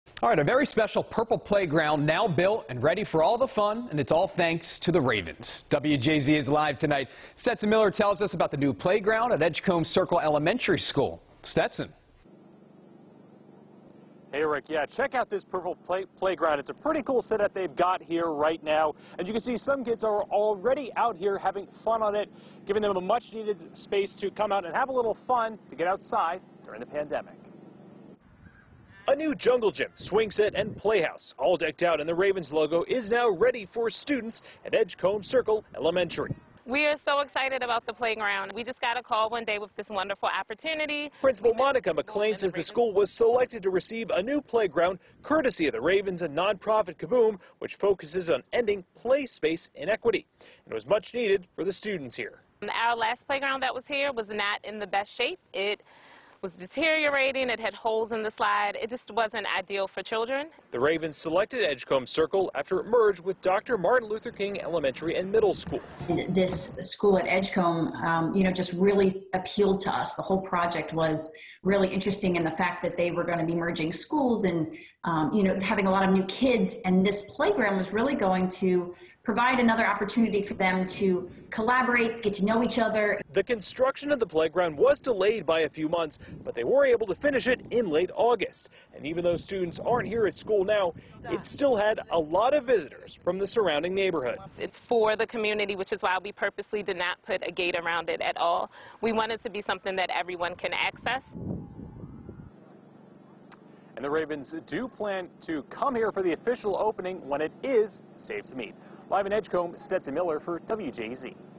Ravens Foundation Helps Build New Playground For Students At Edgecombe Elementary School
A very special purple playground is now built and ready for fun and it’s all thanks to the Ravens.